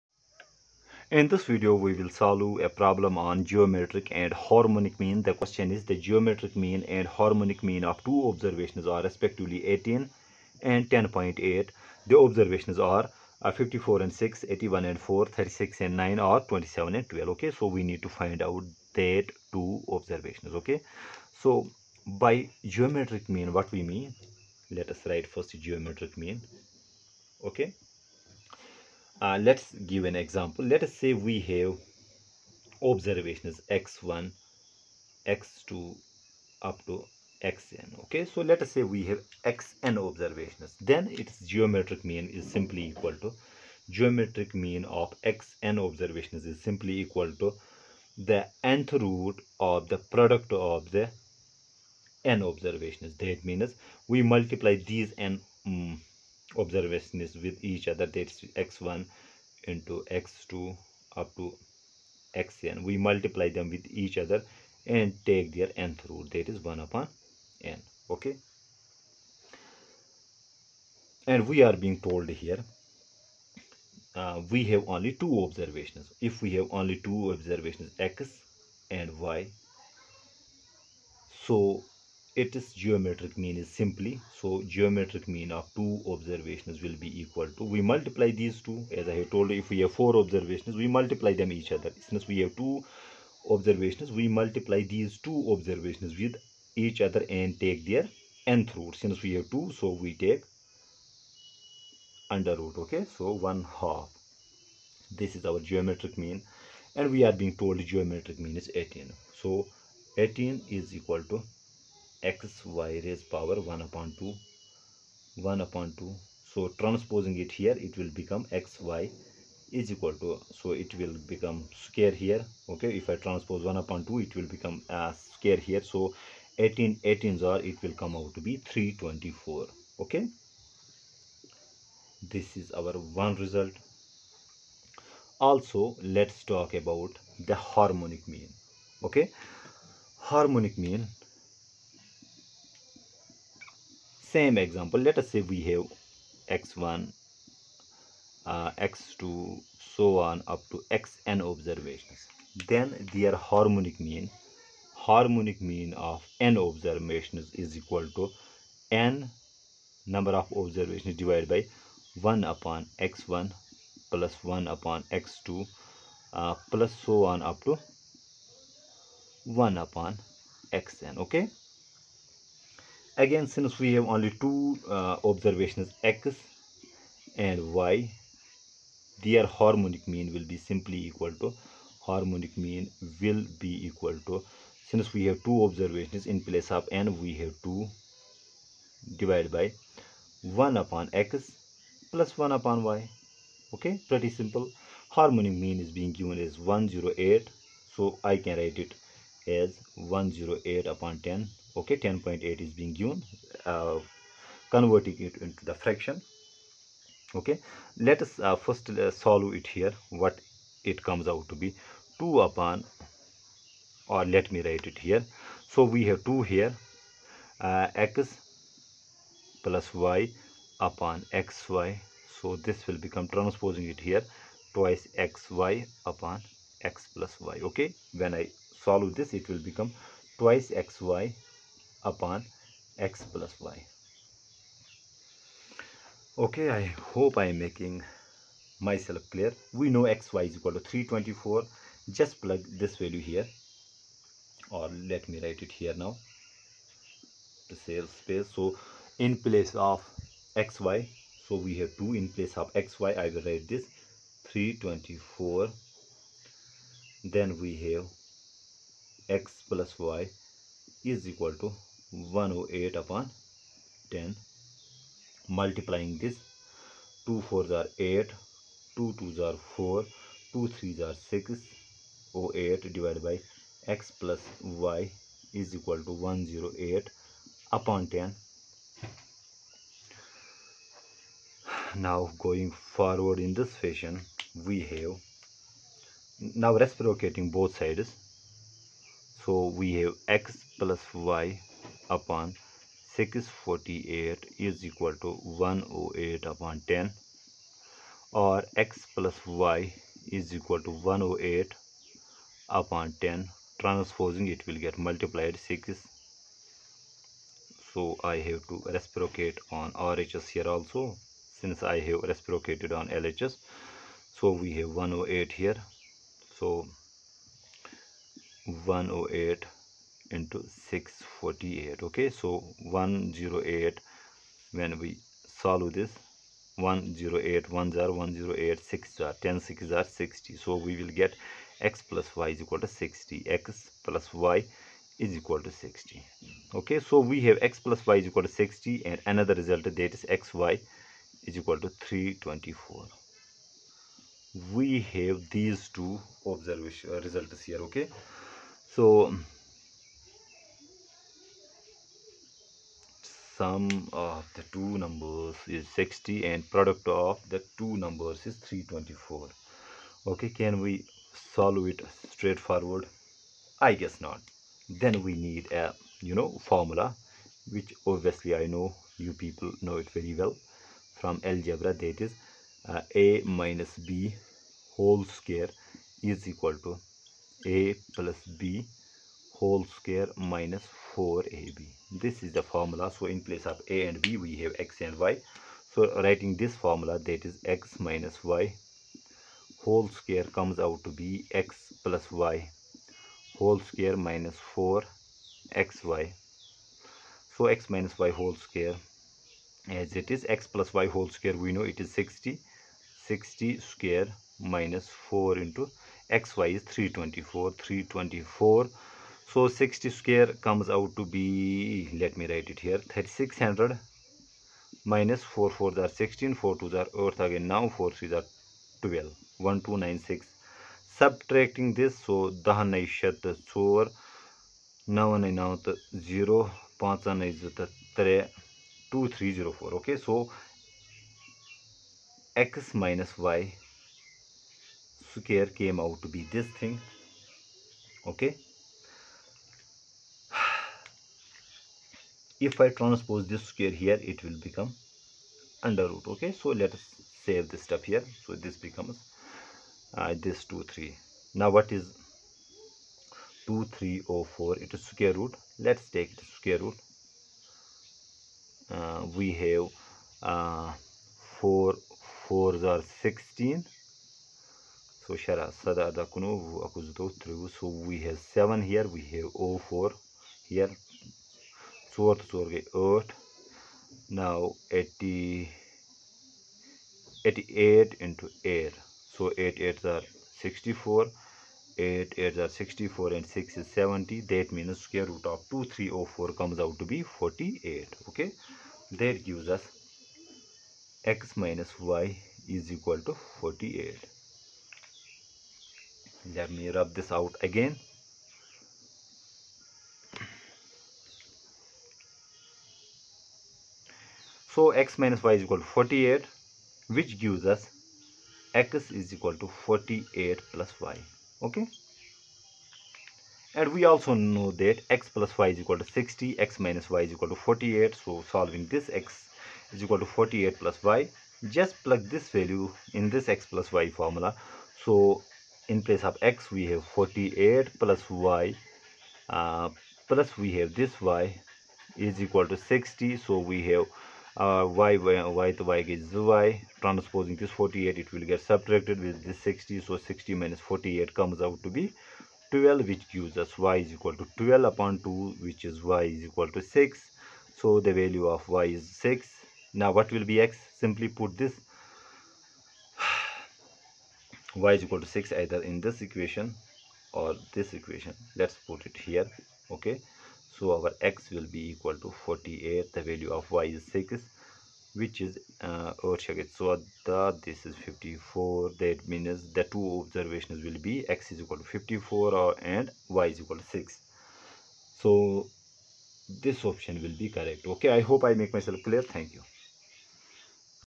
GEOMETRIC MEAN HARMONIC MEAN NUMERICAL. The GM and HMof two observations are respectively 18 & 10.8
The geometricmean and Harmonicmean of two observations are respectively 18 and 10.8.Then the observations are: A) 6and 54 B) 81 and 4 C)36 and 9 D) 27 and 12

IS-LM_MODEL